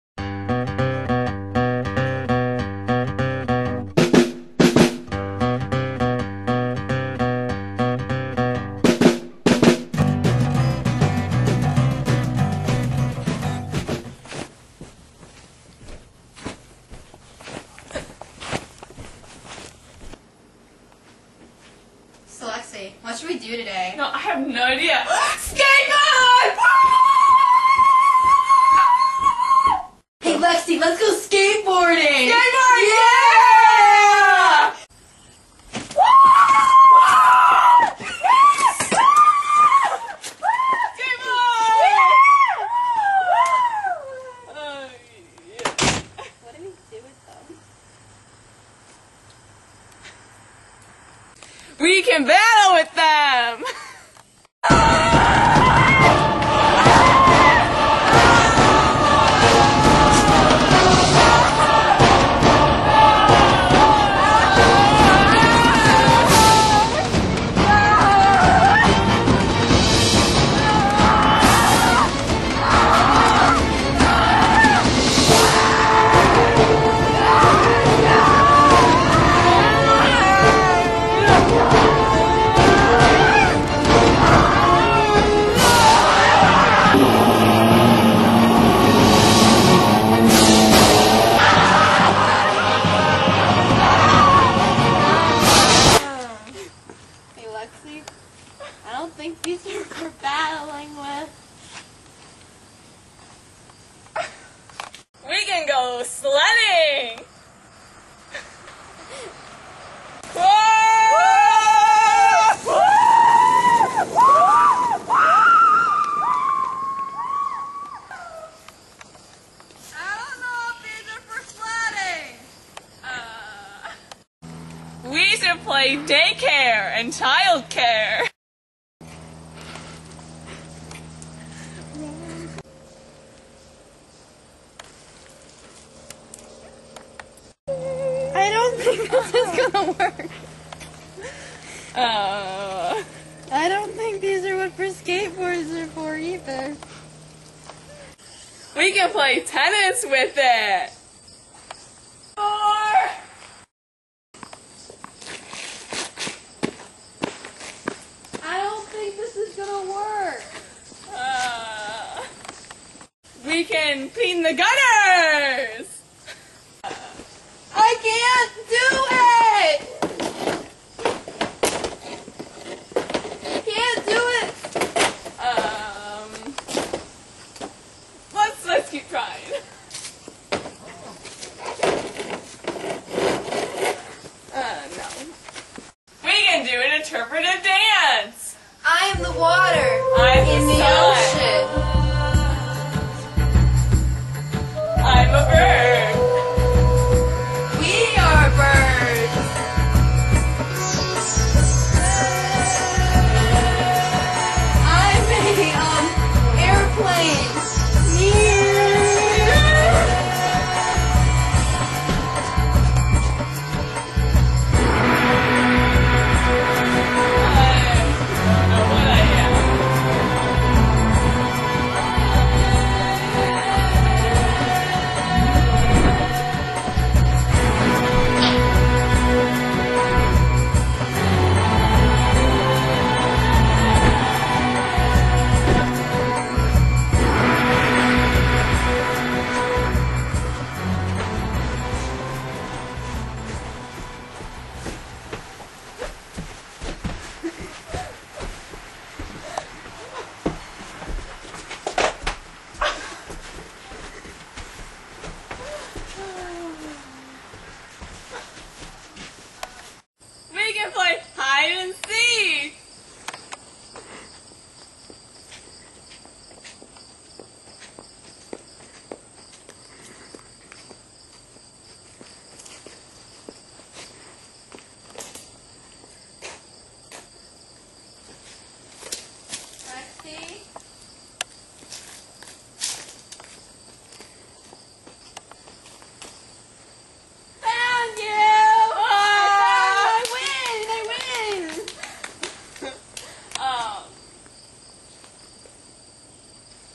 Fall Daze Part 1
The first part in an epic battle of two teens as they struggle to truly understand how one uses a skateboard, or rather, what they really are used for.